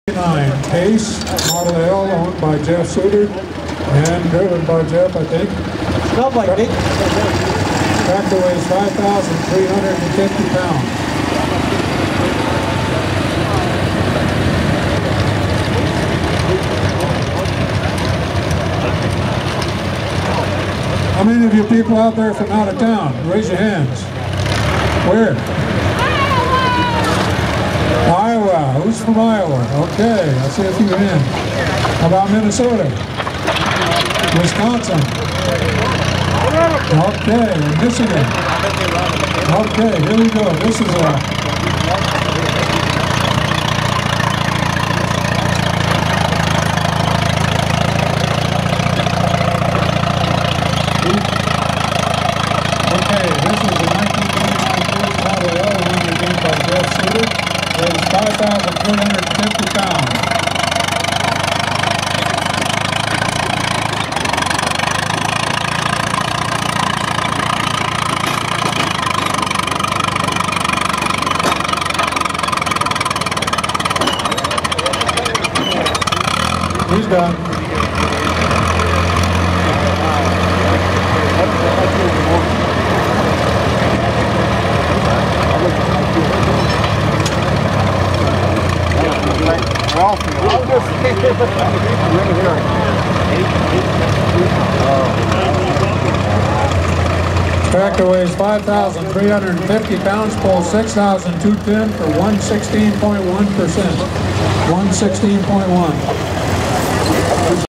1939 Case Model L On Steel Wheels Pulling The Sled At Glendale, AZ 2-8-14
*WATCH IN HD*  I think the announcer is saying 1939...?  This Case Model L is seen at the Arizona Early Day Gas Engine & Tractor Association show at Sahuaro Ranch Park in Glendale, Arizona on February 8th, 2014.